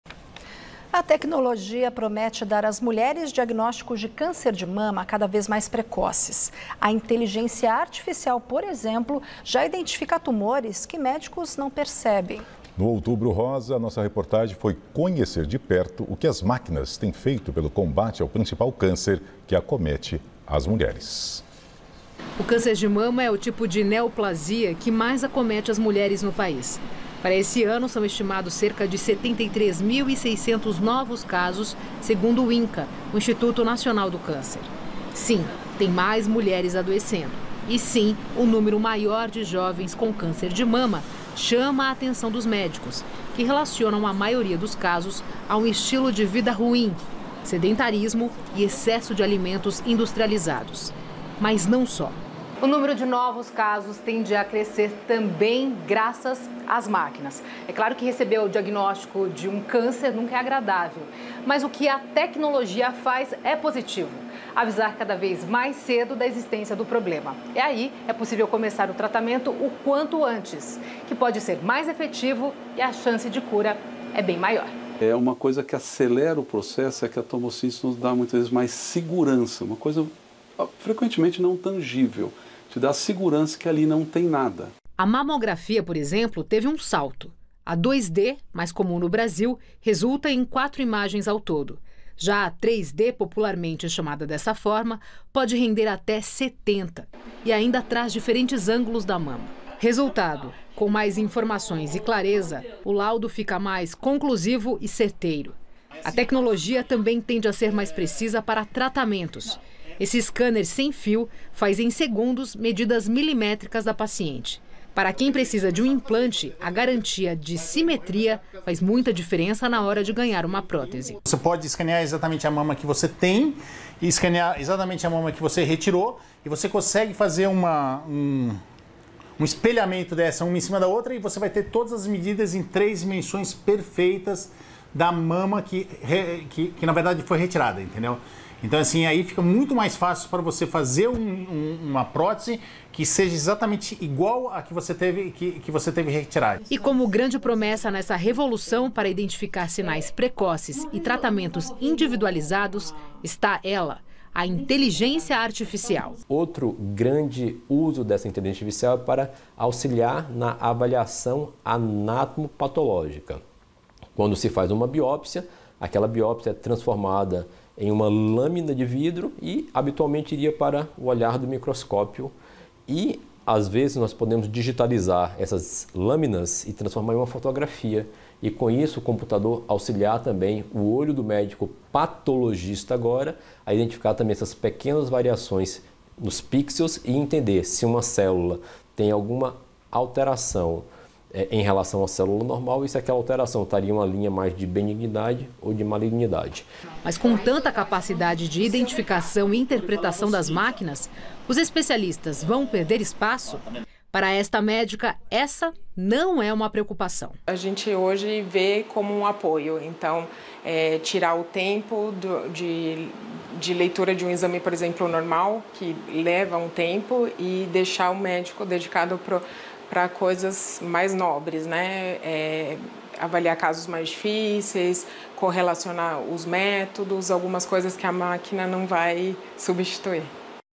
Tecnologia avança contra câncer de mama
A tecnologia promete dar às mulheres diagnósticos de câncer de mama cada vez mais precoces. A inteligência artificial, por exemplo, já identifica tumores que médicos não percebem.

No "outubro rosa", a nossa reportagem foi conhecer o que as máquinas têm feito pelo combate ao principal câncer que acomete as mulheres.